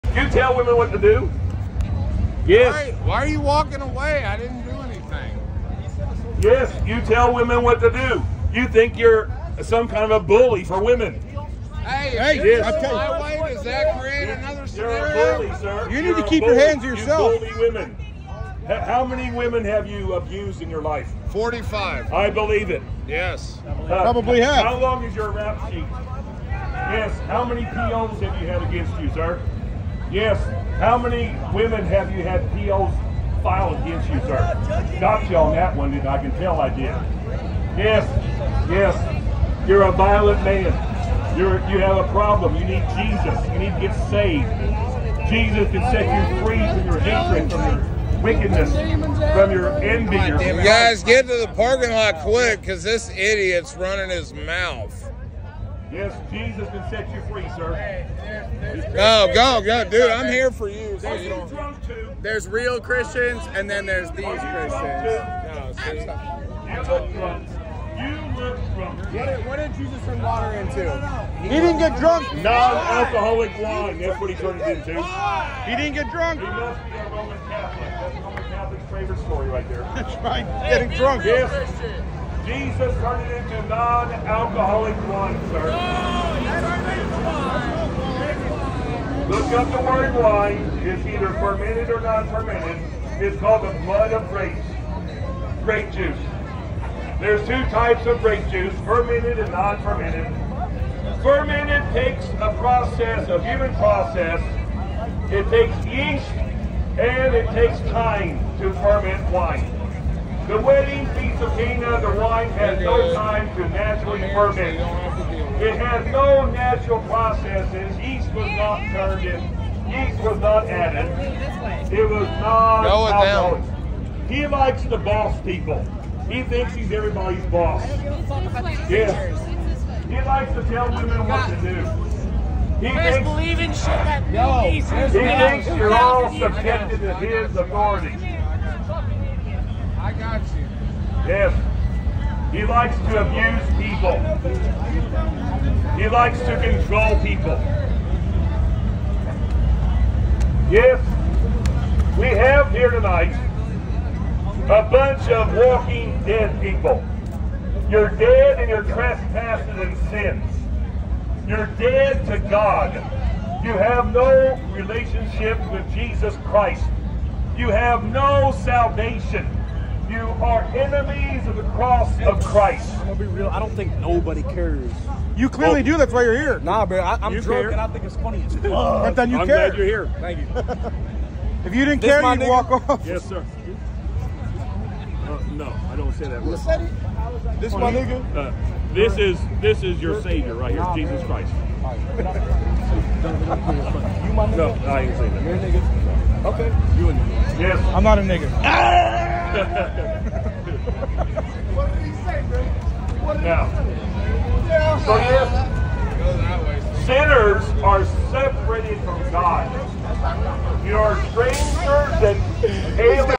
Contentious but Convicted
Contentious but convicted, A very aggressive man with a lot of conviction in his heart at Tulsa Oktoberfest.